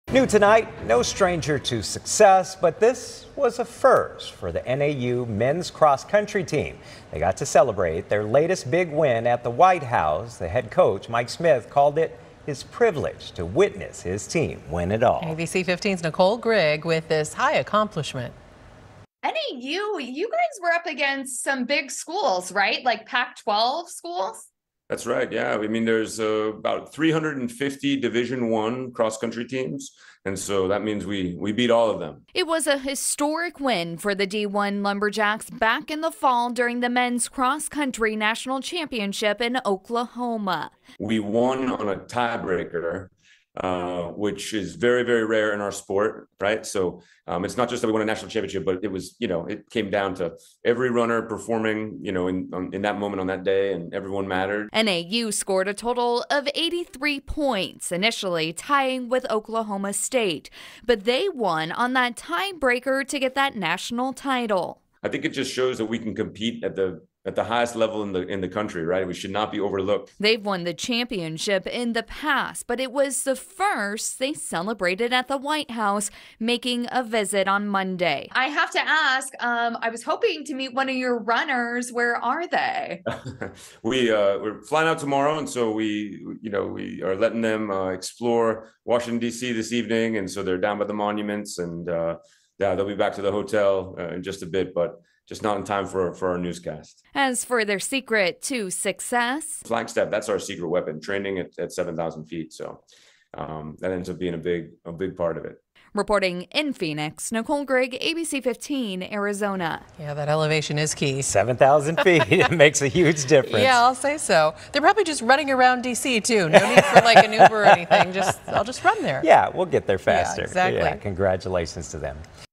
NAU men's cross country team honored by president at White House
The group traveled to Washington D.C. Monday morning to celebrate an historic win at the White House, the team's first-ever visit. At the White House, President Joe Biden hosted both men's and women's NCAA Champion teams from the 2022-23 season to celebrate their achievements.